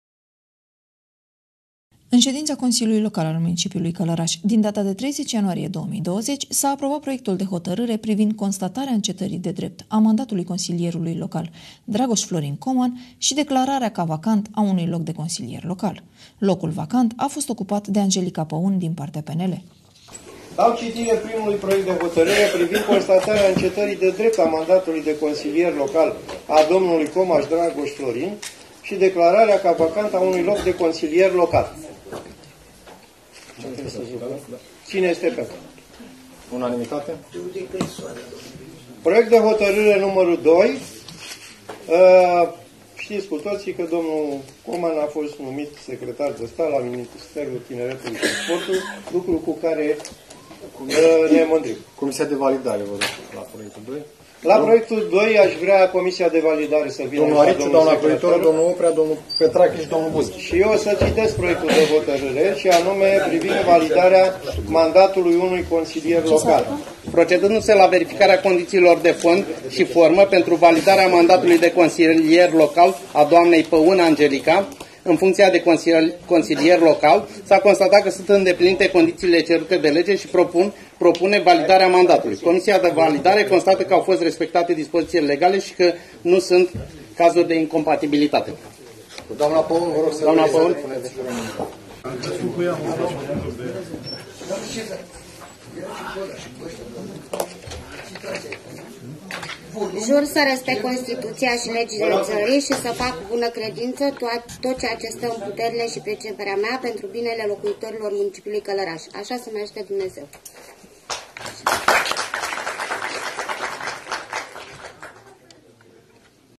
Știri Locale - Angelica Paun, noul consilier local
Ca urmare a încetării mandatului de consilier local al domnului Dragoș Coman, a fost numit un nou consilier în Consiliul Local al Mun. Călărași.